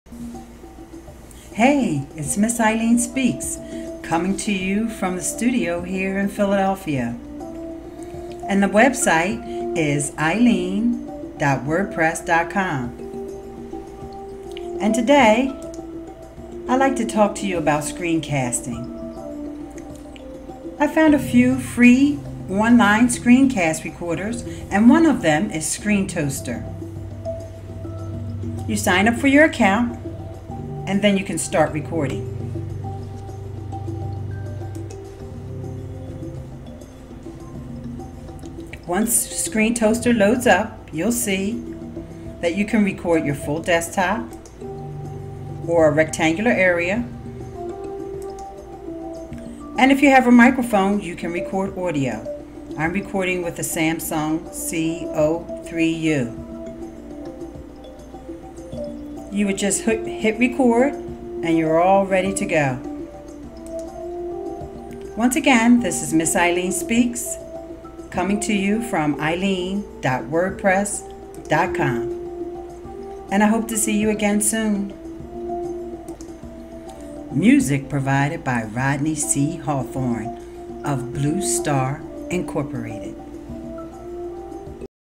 Screencasting 101
Ms. Ileane Speaks shows how easy it is to start making screencasts using Screentoaster.